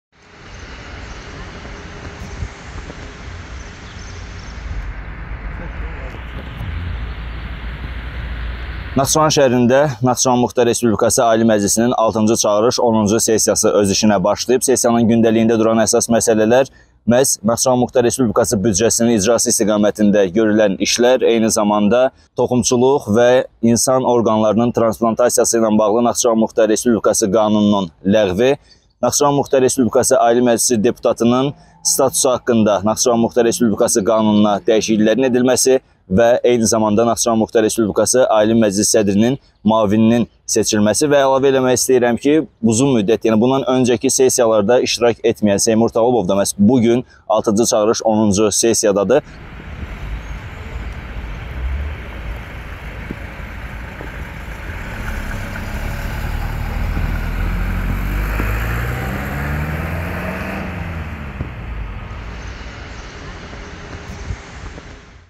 Seymur Talıbov Ali Məclisə niyə gəldi?
Altıncı çağırış Naxçıvan Muxtar Respublikası Ali Məclisinin onuncu sessiyası başlayıb.
Sessiyanın gündəliyinə Naxçıvan Muxtar Respublikasının 2022-ci il büdcəsinin icrasının təsdiq edilməsi, Ali Məclis sədrinin müavininin seçilməsi, “Naxçıvan Muxtar Respublikası Ali Məclisi deputatının statusu haqqında” Naxçıvan Muxtar Respublikasının qanununda dəyişiklik edilməsi, həmçinin "Toxumçuluq haqqında” və “İnsan orqan və ya toxumalarının transplantasiyası haqqında” Naxçıvan Muxtar Respublikası qanunlarının ləğv edilməsi məsələləri daxil edilib.
Sessiyada Naxçıvan Muxtar Respublikası Ali Məclisinin sabiq sədri Vasif Talıbovun oğlu, 25 saylı Naxçıvan şəhər Seçki Dairəsindən seçilmiş Seymur Talıbov da iştirak edir.